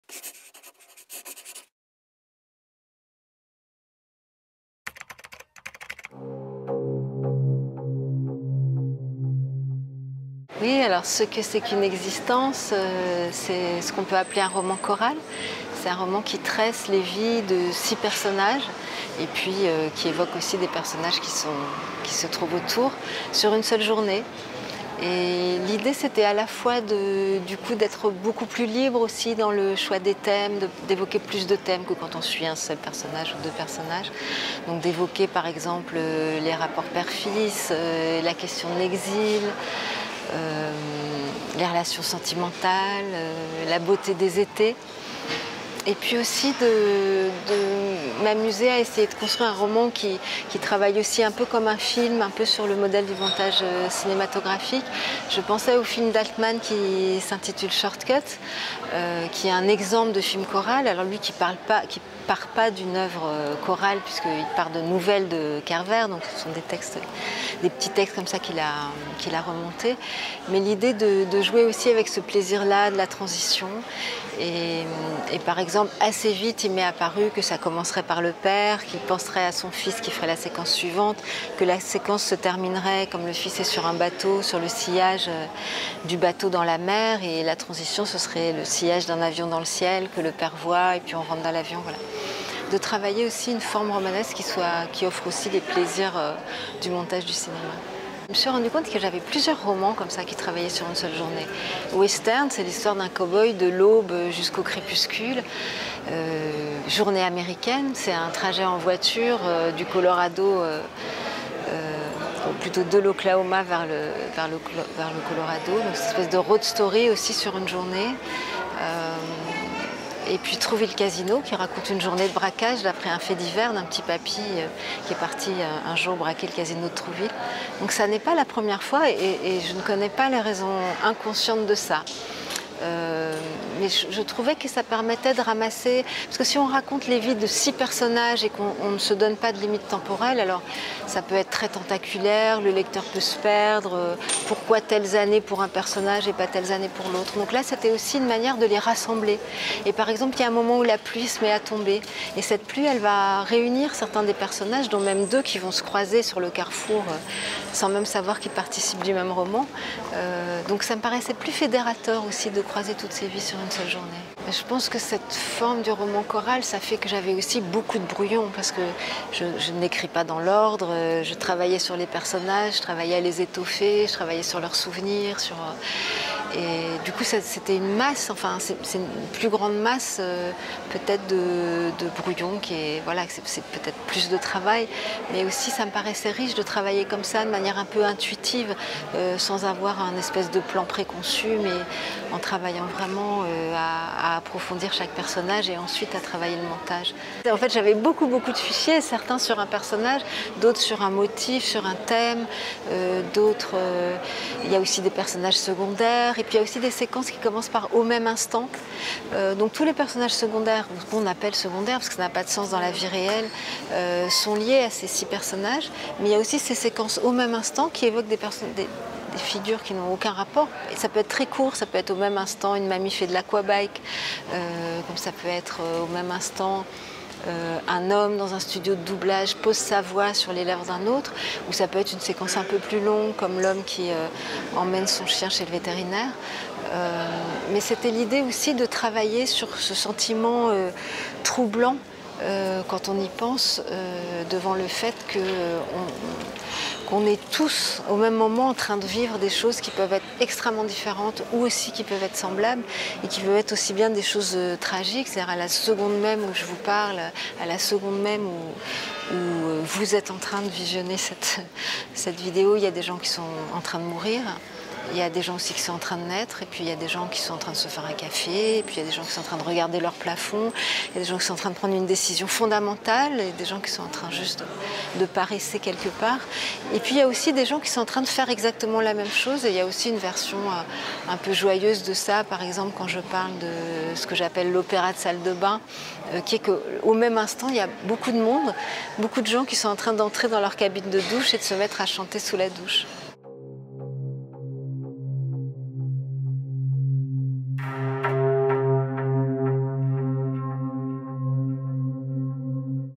Christine Montalbetti - Ce que c'est qu'une existence
A l'occasion des Correspondances de Manosque 2021, Christine Montalbetti vous présente son ouvrage "Ce que c'est qu'une existence" aux éditions POL . Rentrée littéraire automne 2021.